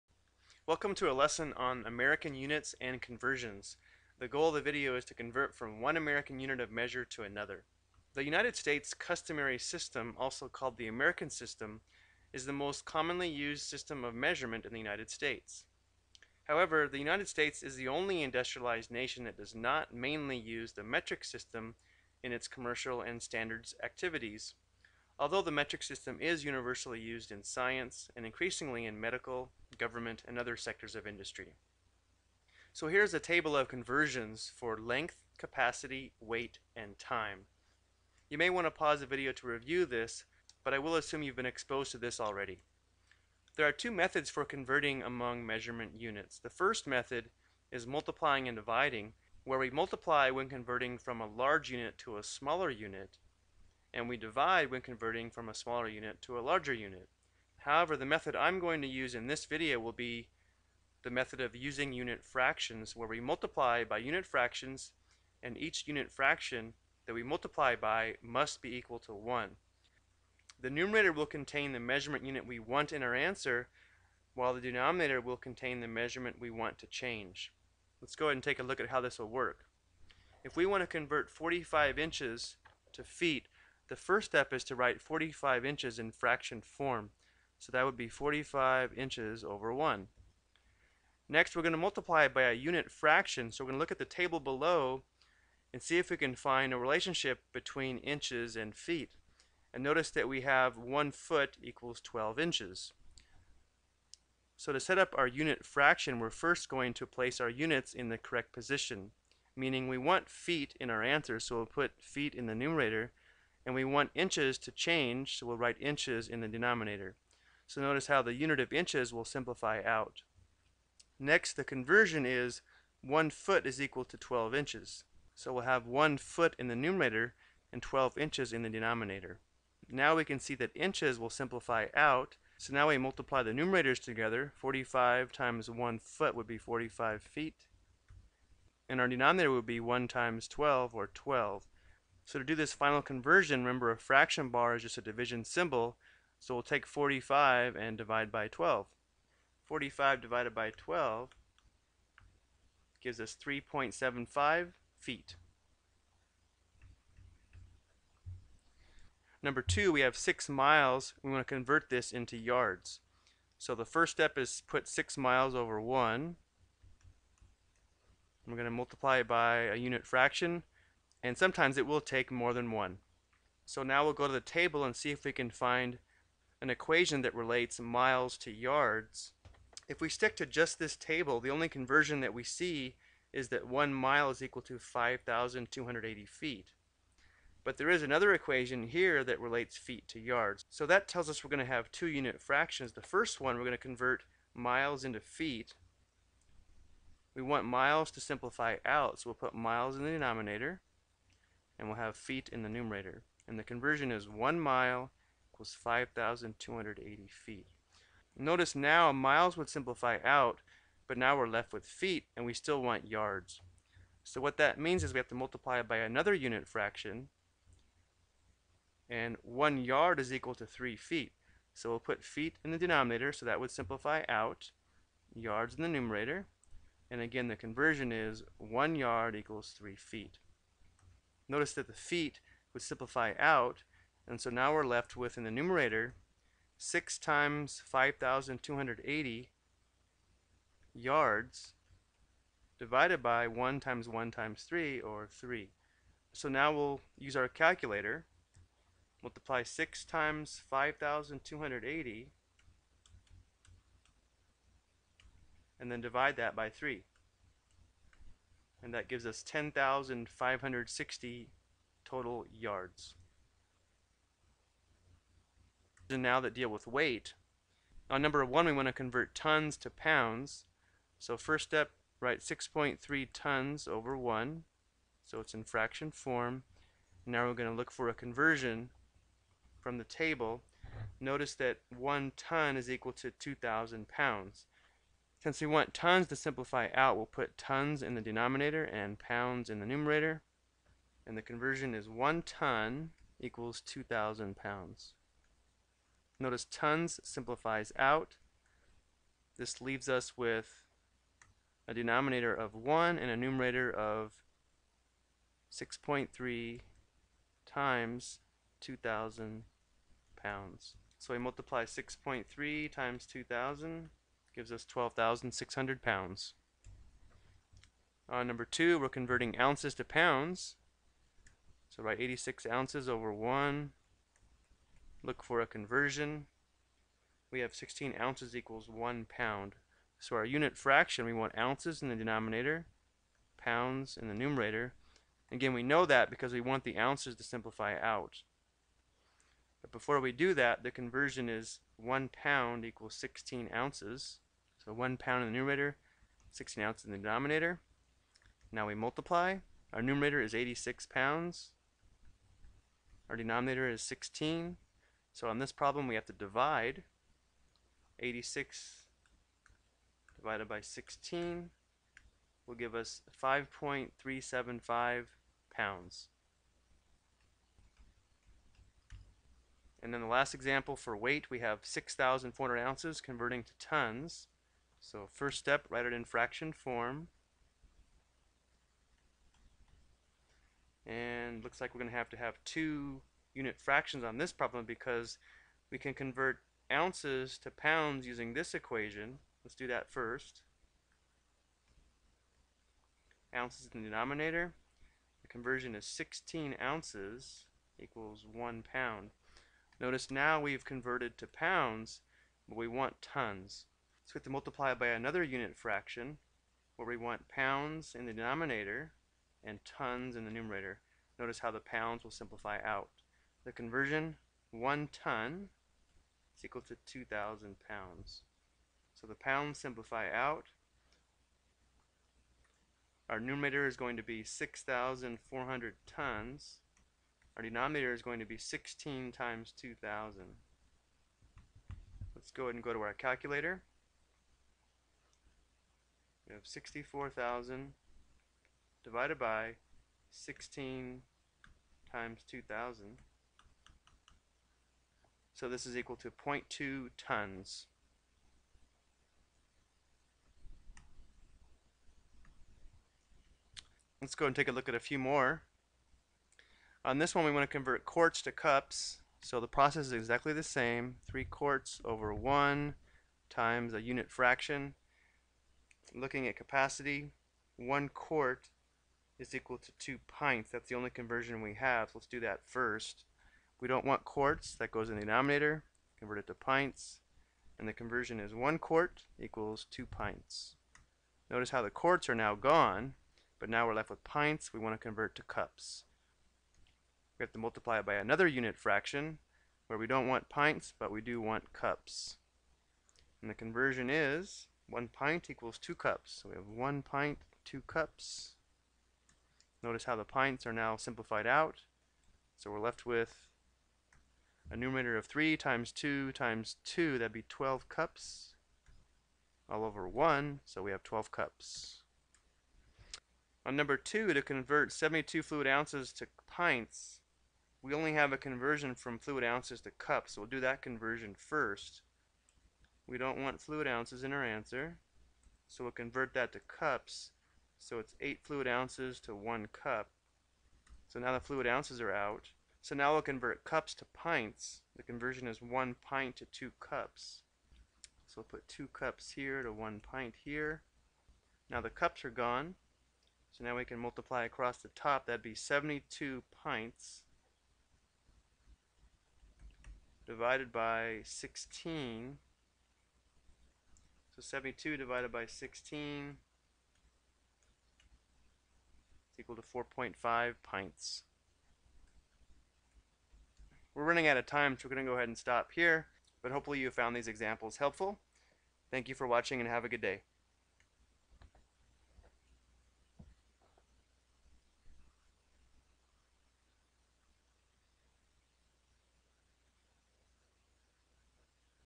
American Unit Conversion
This video explains how to convert between different American or Standard unit of measure for length, capacity, and weight.